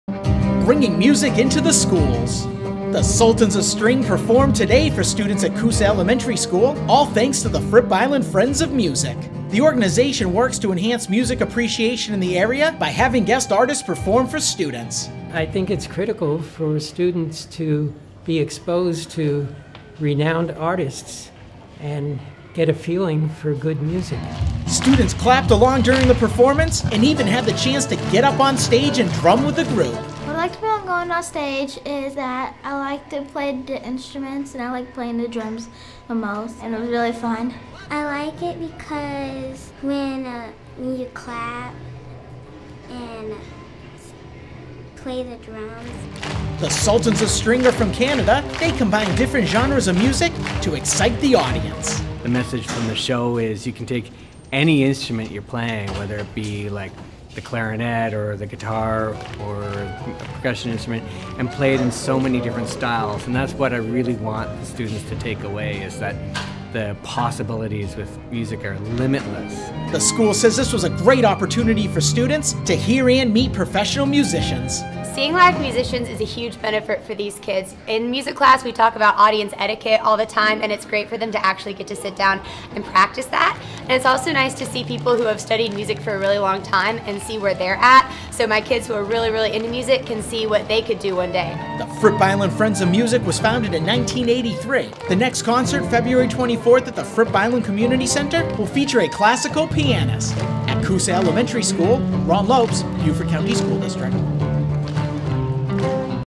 Professional Musicians Perform for Students
The Fripp Island Friends of Music brought the Sultans of String to Coosa Elementary School to perform for the students.